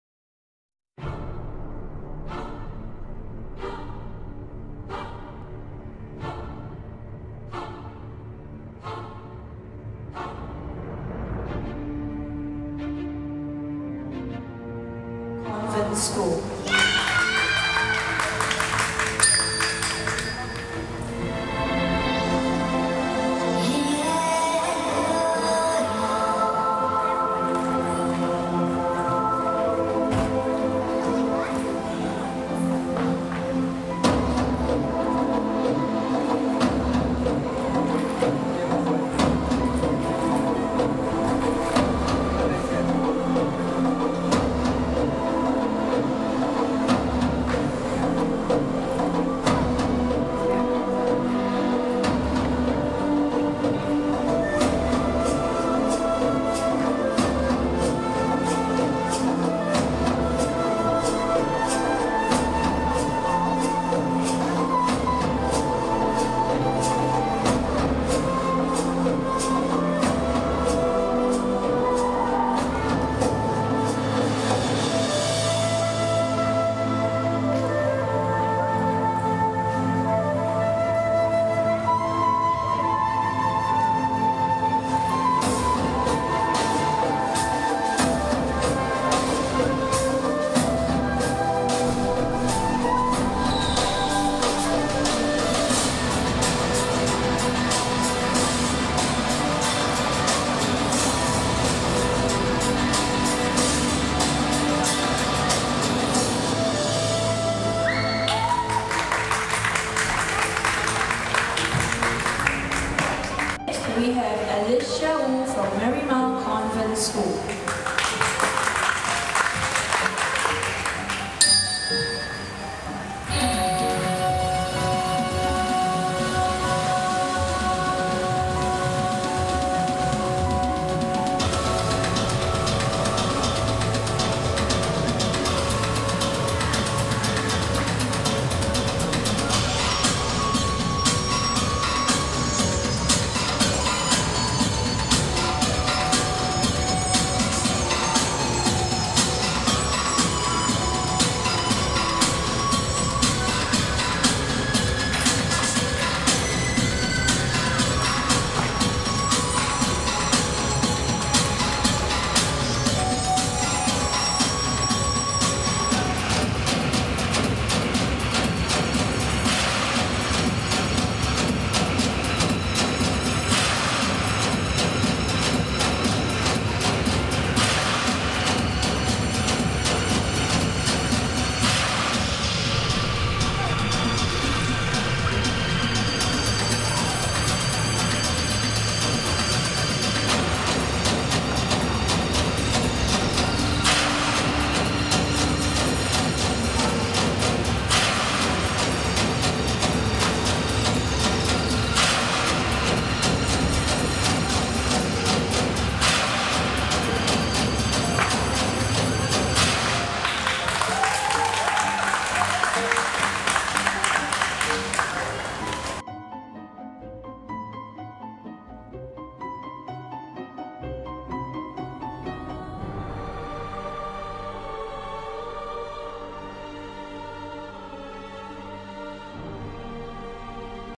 National School Games Rhythmic Gymnastics Championships 2016 Alyssia Woo by Jeffini Photography
Video by Jef Choo
National School Games Rhythmic Gymnastics Championships 2016 was held in Singapore on 12 to 15Apr 2016 at Bishan Sports Hall.
Feature:  Gymnast Alyssia Woo RG Level 3